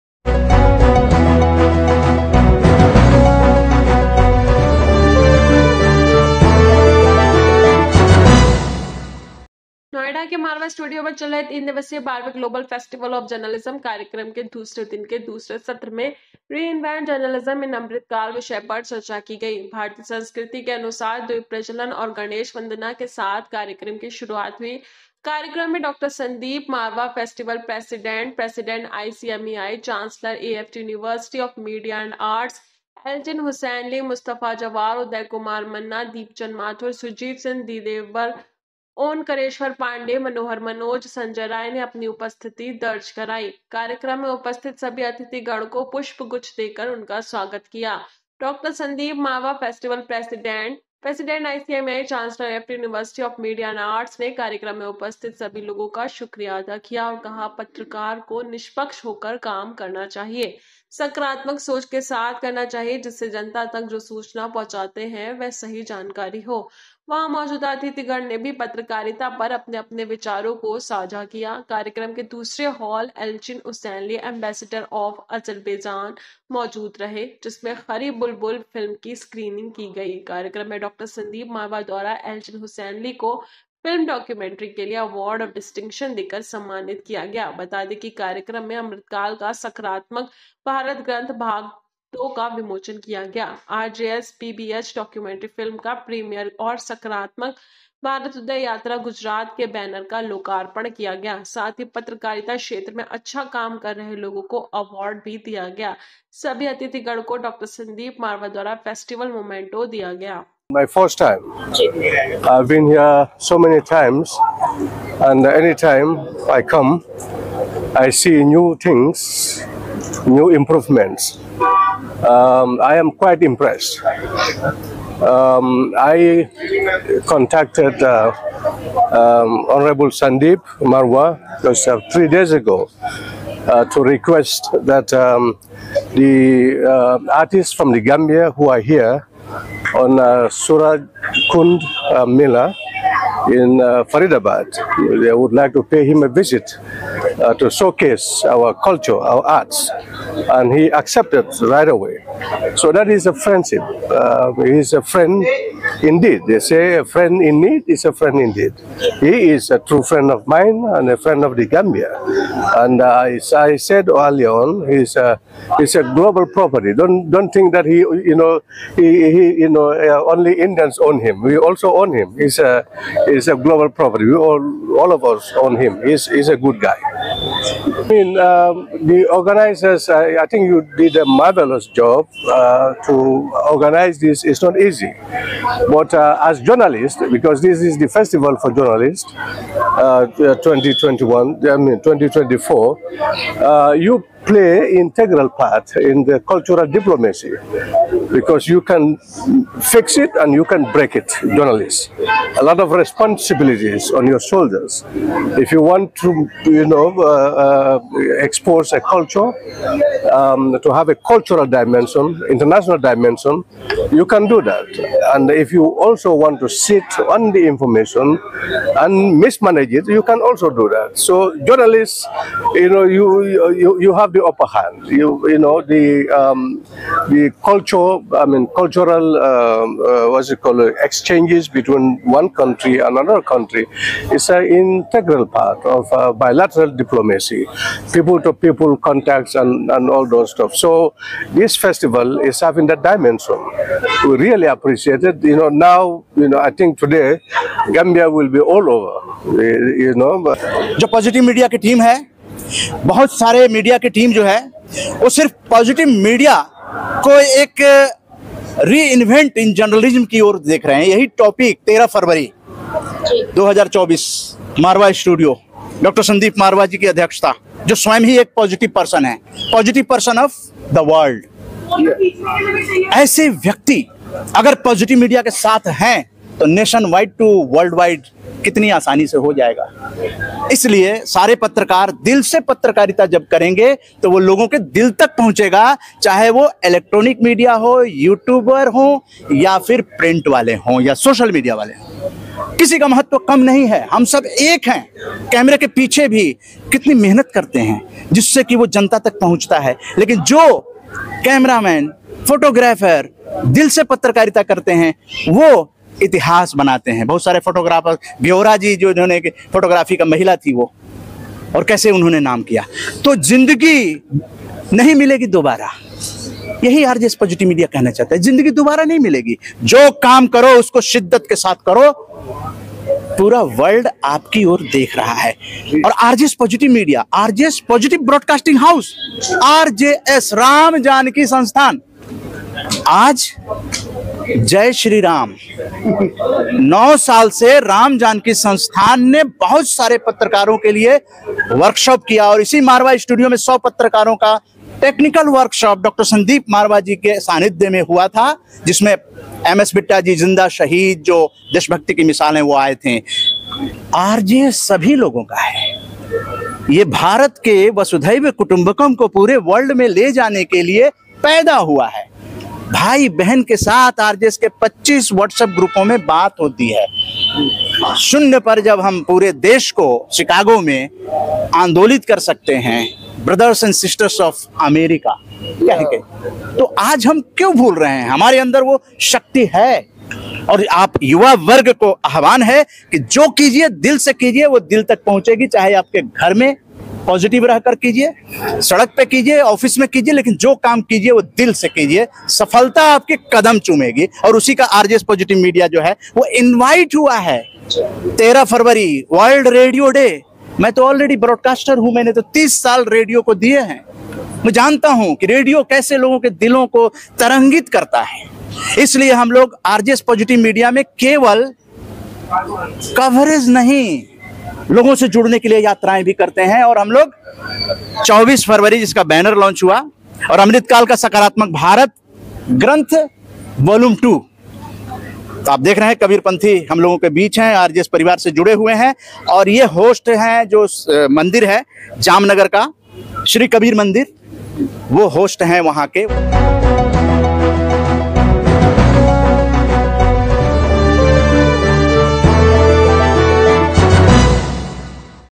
मारवाह स्टूडियों में दूसरे दिन के दूसरे सत्र में Reinvent Journalism In Amrit Kaal 'पर चर्चा!
नोएडा के मारवाह स्टूडियों में चल रहे तीन दिवसीय  '12 वें ग्लोबल फेस्टिवल ऑफ जर्नलिज्म ' कार्यक्रम के दूसरे दिन के दूसरे सत्र में ' Reinvent Journalism In Amrit Kaal 'विषय पर चर्चा की गई। भारतीय संस्कृति के अनुसार द्वीप प्रज्वलन और गणेश वंदना के साथ कार्यक्रम की शुरुआत हुई। कार्यक्रम में डॉ संदीप मारवाह , ( फेस्टिवल प्रेसिडेंट , प्रेसिडेंट ICMEI, चांसलर , एएएफटी यूनिवर्सिटी ऑफ मीडिया एंड ऑर्ट्स ) , एलचिन हुसैनली ( एंबेसडर ऑफ अजरबैजान ) , मुस्तफा जवारा (हाई कमीशन ऑफ गम्बिया ) , उदय कुमार मन्ना (संस्थापक एवं राष्ट्रीय संयोजक , आरजेएस पॉजिटिव मीडिया ) , दीपचंद माथुर , सुरजीत सिंह दीदेवर , ओनकरेश्वर पांडेय (सीनियर जर्नलिस्ट ) , मनोहर मनोज (सीनियर जर्नलिस्ट ) , संजय राय (सीनियर जर्नलिस्ट ) ने अपनी उपस्थिति दर्ज कराई।
कार्यक्रम में उपस्थित सभी अतिथिगण को 'पुष्पगुच्छ ' देकर उनका स्वागत किया ।
डॉ संदीप मारवाह (फेस्टिवल प्रेसिडेंट , प्रेसिडेंट ICMEI, चांसलर , एएएफटी यूनिवर्सिटी ऑफ मीडिया एंड ऑर्ट्स ) ने कार्यक्रम में उपस्थित सभी लोगों का शुक्रिया अदा किया और कहा, पत्रकार को  निष्पक्ष होकर काम करना चाहिए , सकारात्मक सोच के साथ करना चाहिए जिससे जनता तक जो सूचना पहुंचाते है वह सही जानकारी हो।
वहाँ मौजूदा अतिथिगण ने भी पत्रकारिता पर अपने - अपने विचारों को साझा किया।
कार्यक्रम के दूसरे हॉल एलचिन हुसैनली (एंबेसडर ऑफ अजरबैजान ) मौजूद रहे। जिसमें ' Khari Bulbul 'फिल्म की स्क्रीनिंग की गयी। कार्यक्रम में डॉ संदीप मारवाह द्वारा एलचिन हुसैनली को फिल्म डॉक्यूमेंट्री के लिए Award of distinction देकर सम्मानित किया गया।
बता दें कि कार्यक्रम में 'अमृतकाल का सकारात्मक भारत ग्रंथ भाग 2 ' का विमोचन किया , आरजेएस पीबीएच डॉक्यूमेंट्री फिल्म का प्रीमियर और सकारात्मक भारत उदय यात्रा गुजरात के बैनर का लोकार्पण किया गया । साथ ही पत्रकारिता क्षेत्र में अच्छा काम रहे लोगों को अवार्ड भी दिया गया।